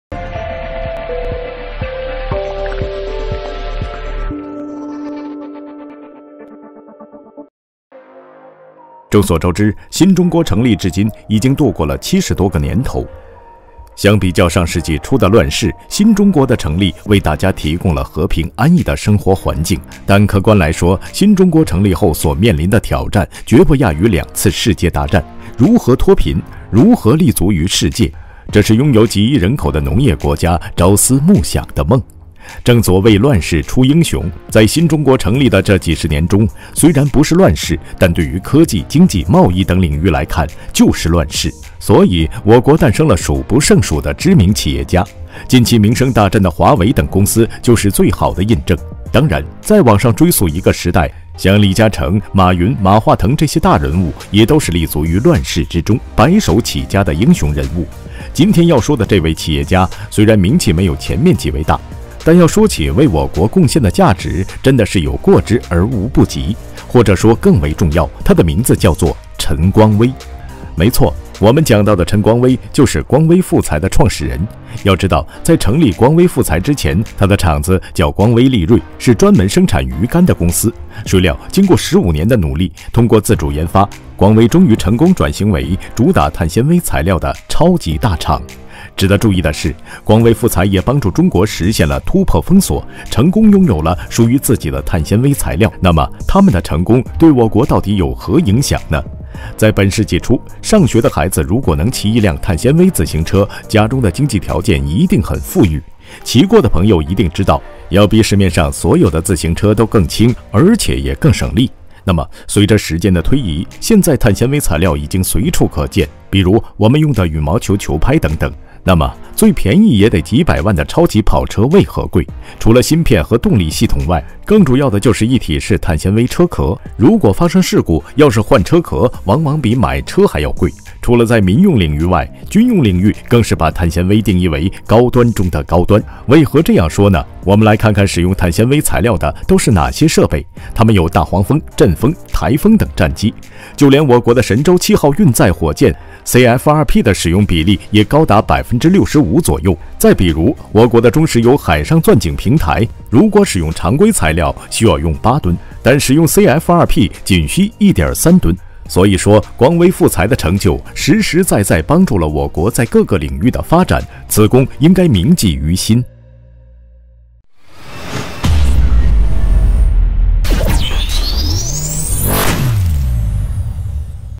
國之驕傲！從做魚竿起家，用了15年的時間成為世界碳纖維巨頭（Pride of the country! Starting from making fishing rods, it took 15 ）
眾所周知，新中國成立至今已經度過了70多個年頭，相比較上世紀初的亂世，新中國的成立為大家提供了和平安逸的生活環境。但客觀來說，新中國成立後所面臨的挑戰絕不亞於兩次世界大戰，如何脫貧？如果立足於世界？（As we all know, more than 70 years have passed since the founding of New China. Compared with the troubled times at the beginning of the last century, the founding of New China has provided everyone with a peaceful and peaceful living environment. But objectively speaking, the challenges faced by the People’s Republic of China after the founding of the People’s Republic of China are no less than the two world wars. How to get rid of poverty? If it is based on the world?）